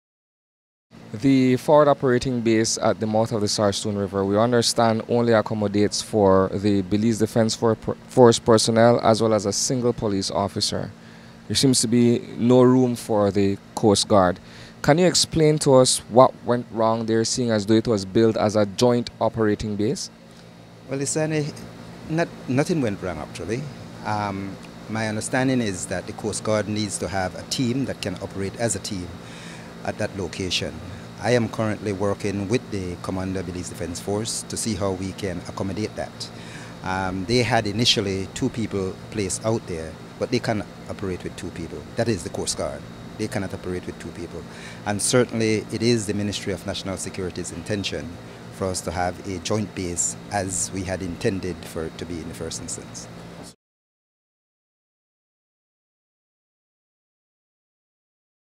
No Room for Coast Guard at Sarstoon F.O.B.
The forward operating base at the estuary of the Sarstoon River was erected as a joint military installation to house Belize Defense Force and coast guard personnel. There was an official ceremony with all the bells and whistles to launch it in April 2016.  The F.O.B. was intended to be used to support tactical operations within the troublesome area where the Guatemalan military has unimpeded access.  While the objective remains integral, three months after its launch, the base is only being utilized by the B.D.F.  That’s because the facility is too small and there is presently no room for additional manpower at that location.  As it stands, the facility is only giving shelter to eight soldiers and a police officer, leaving the coastguard seamen out of the picture.  According to Retired Colonel George Lovell, C.E.O. in the Ministry of National Security, there is a plan being put together to see how best a coastguard team can be accommodated at the F.O.B.